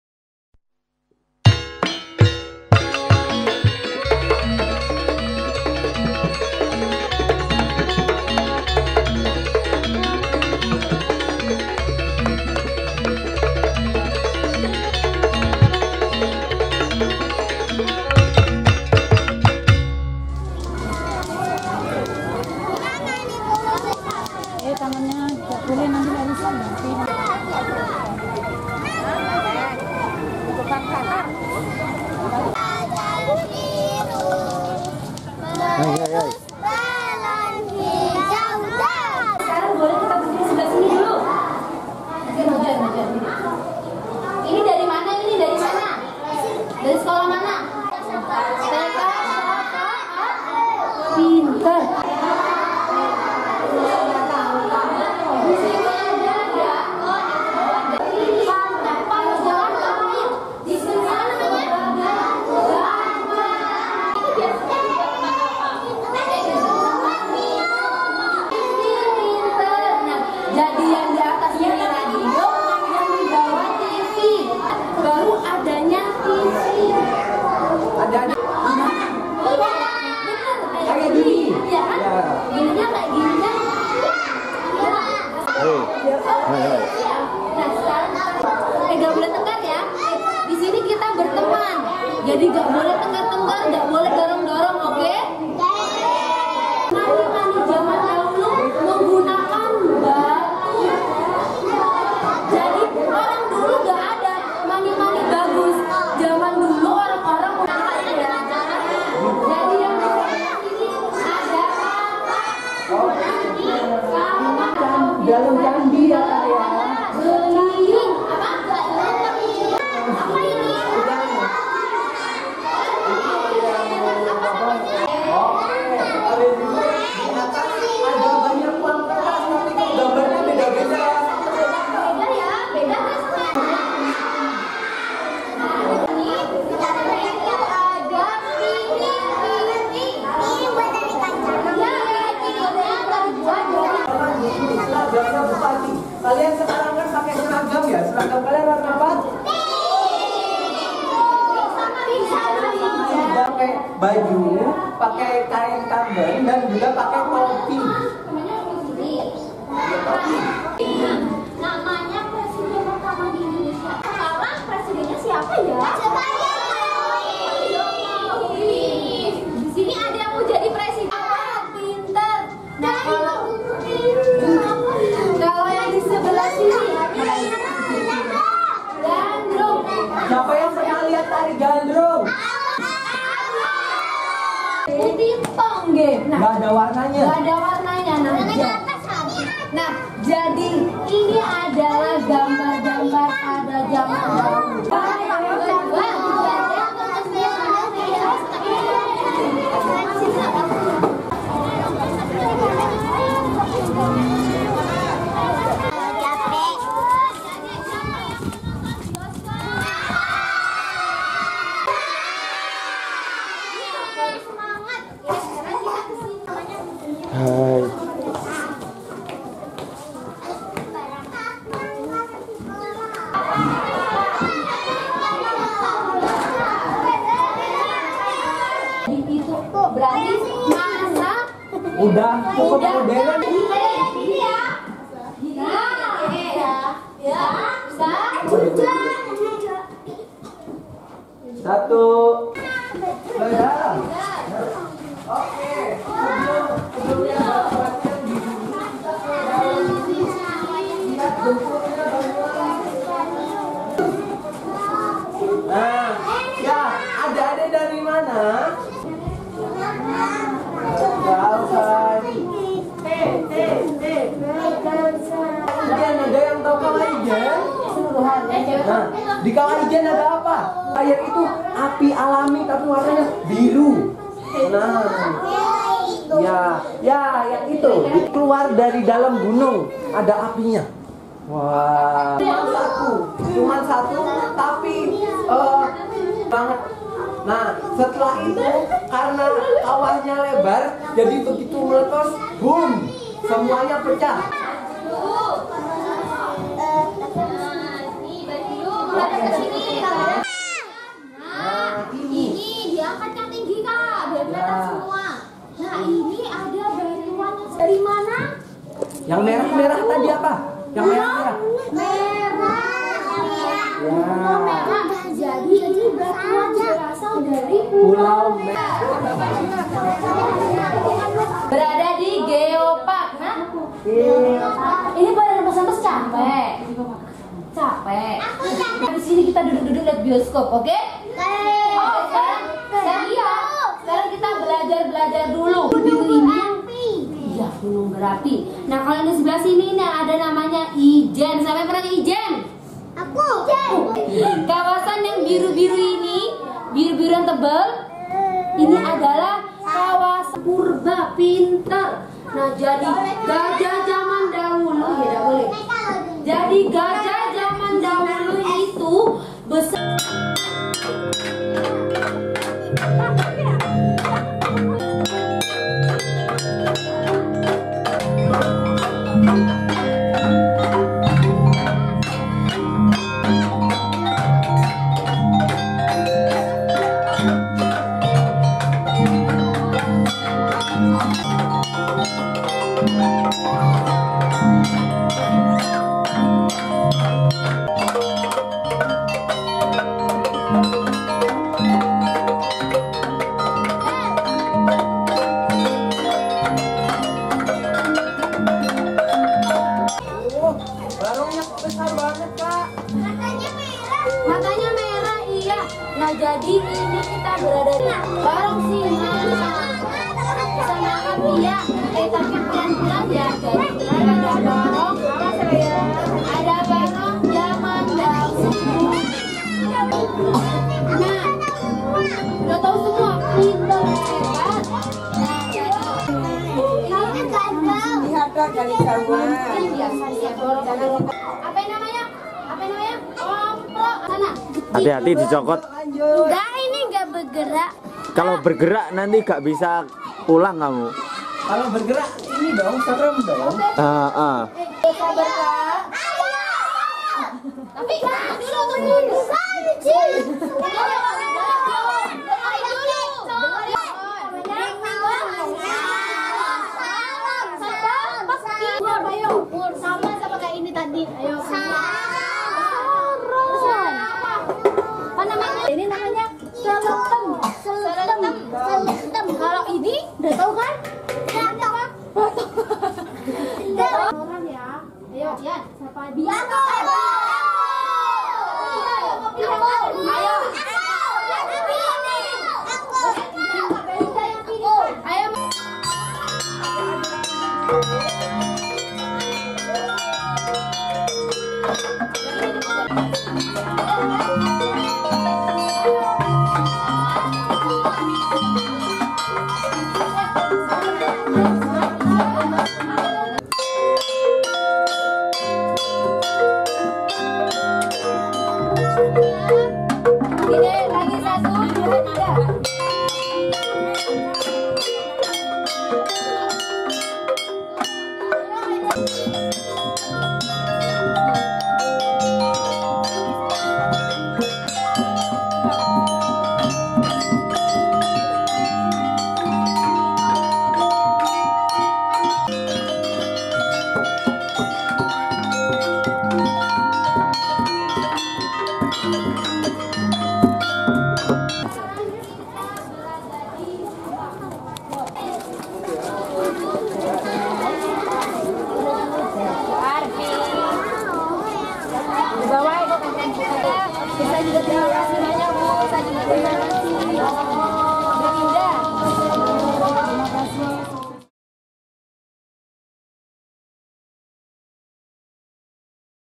Ayo Ke Museum - Kunjungan PAUD/TK Muhtar Syafa'at Blokagung Banyuwangi 2023
Kunjungan PAUD/TK Muhtar Syafa'at Blokagung Banyuwangi Ke Museum Blambangan banyuwangi Kamis 2 Februari 2023